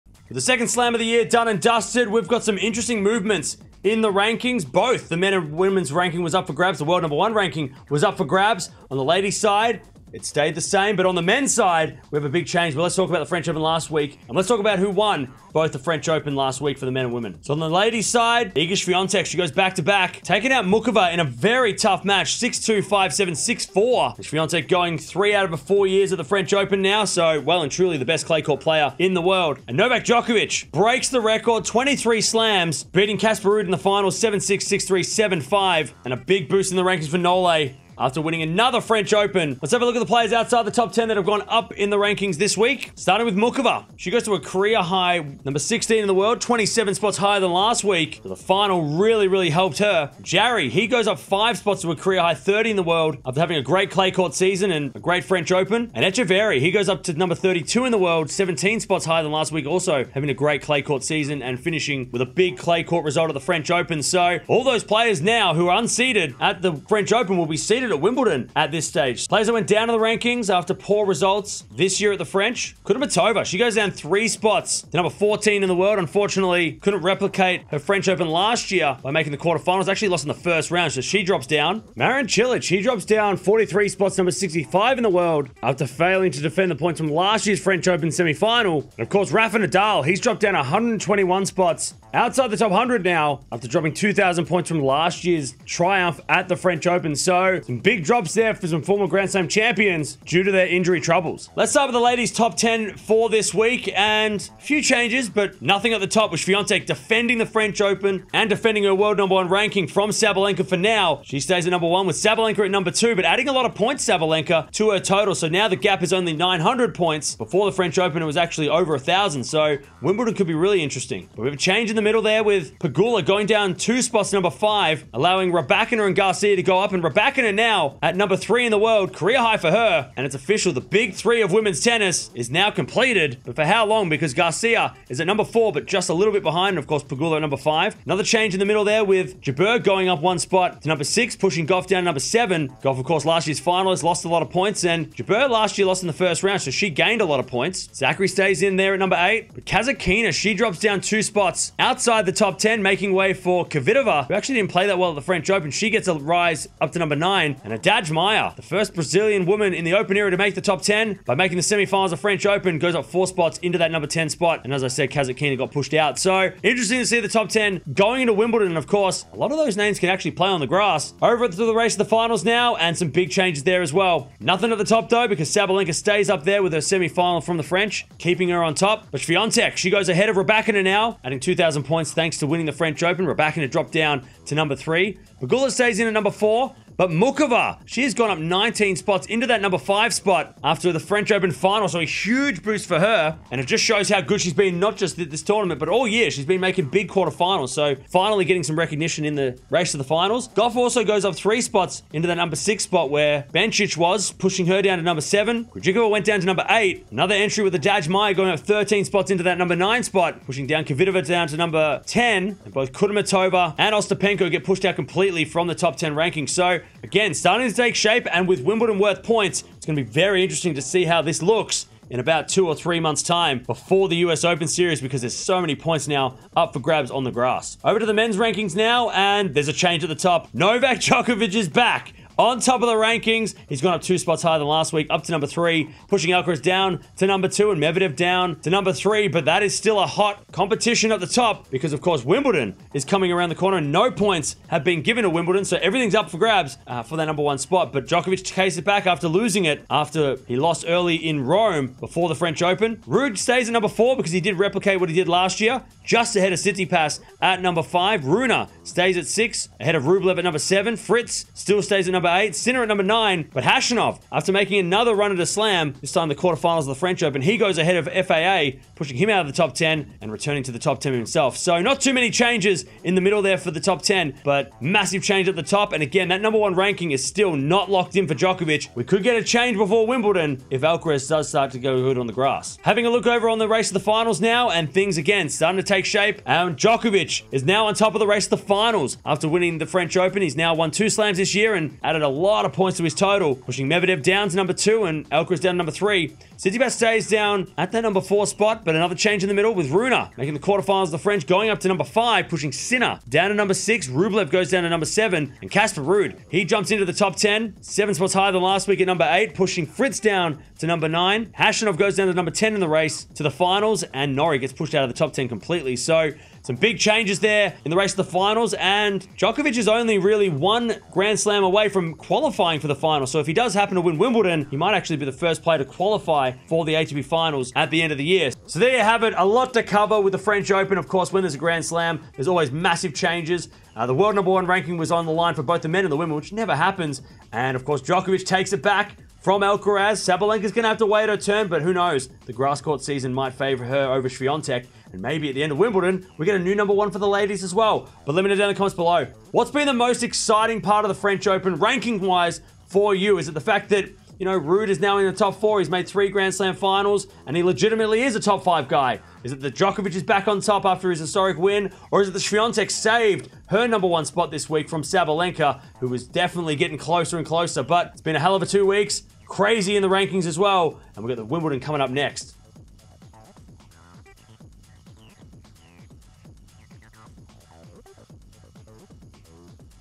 Djokovic, Swiatek Triumph at French Open 2023 | Nadal, Alcaraz Drop | Tennis Rankings
After Roland Garros 2023 is now over with Novak Djokovic and Iga Swiatek taking the titles and securing their place at the top of the rankings, we now move focus to Wimbledon 2023 where points have been restored and Russian player ban has been lifted. Djokovic has taken back top spot over Alcaraz while Swiatek holds Sabalenka off for now. Also Muchova, Haddad Maia and Ruud receive big boosts to their Tour Finals ranks.

0:00 | Intro
0:18 | Past Results
0:40 | Ups & Downs
1:42 | WTA Rankings
2:52 | WTA Finals Race
3:48 | ATP Rankings
4:43 | ATP Finals Race
5:28 | Outro